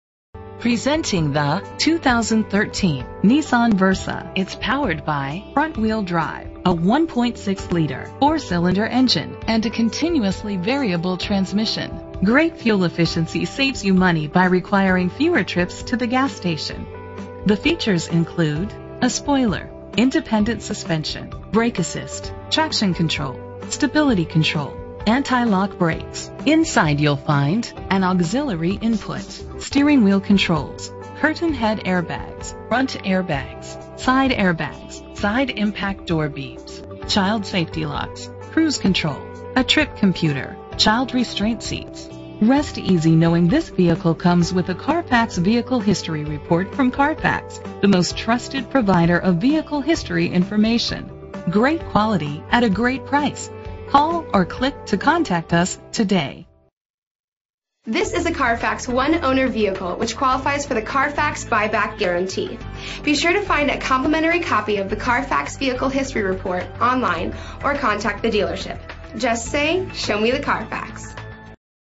2013 Nissan Versa 60367A - Corinth TX
Year: 2013
Make: Nissan
Model: Versa
Trim: 1.6 SV
Engine: 1.6 liter 4 Cylindercylinder FWD
Transmission: CVT
Color: Dk. Gray
Mileage: 91645
Address:
VIN:3N1CN7AP5DL858711